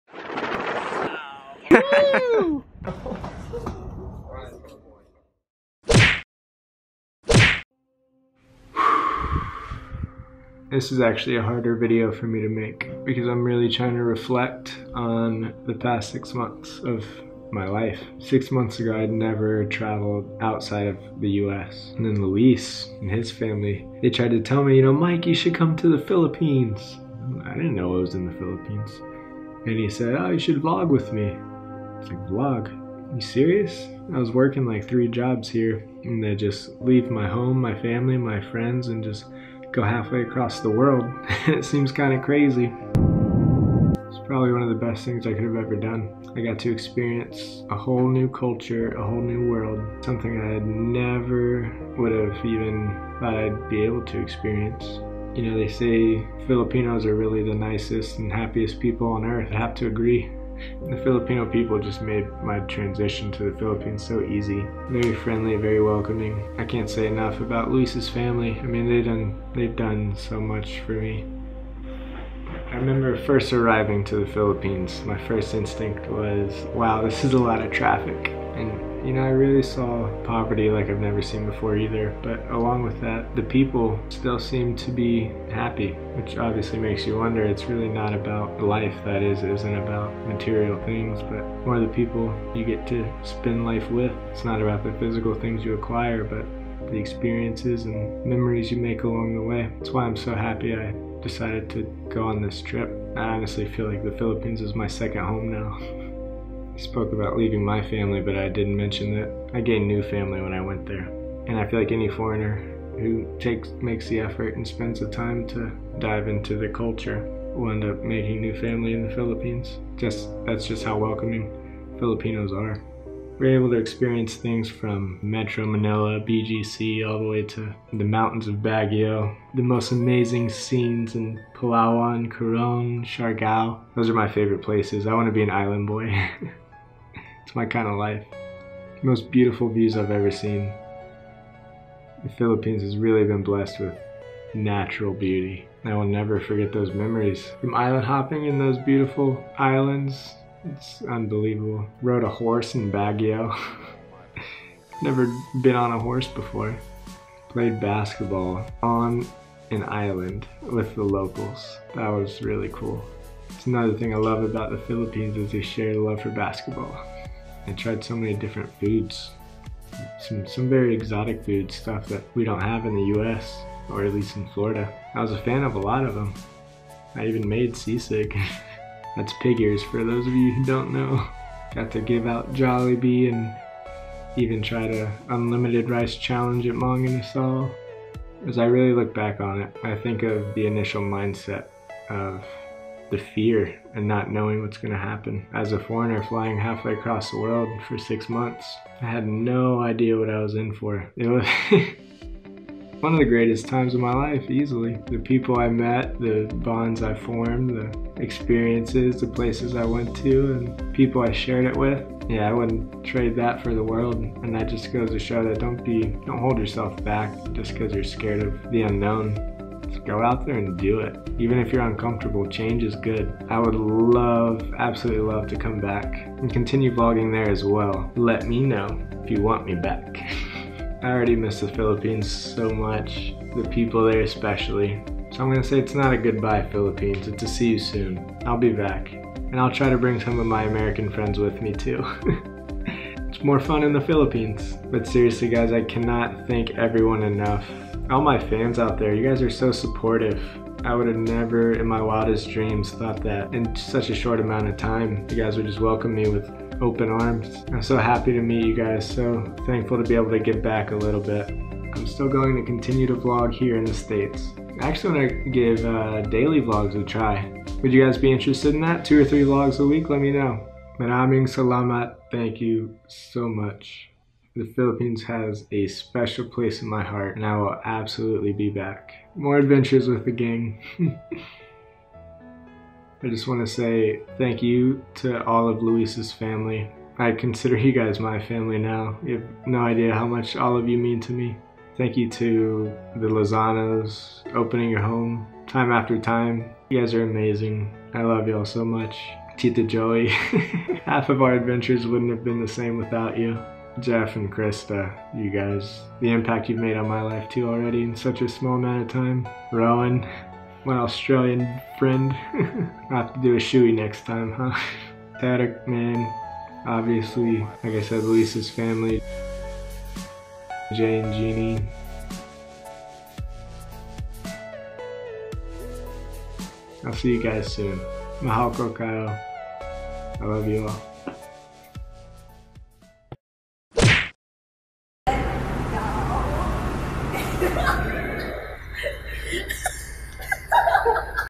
Goodbye Philippines (see you soon)
Thank you for the best 6 months I could've asked for, Philippines. See you soon.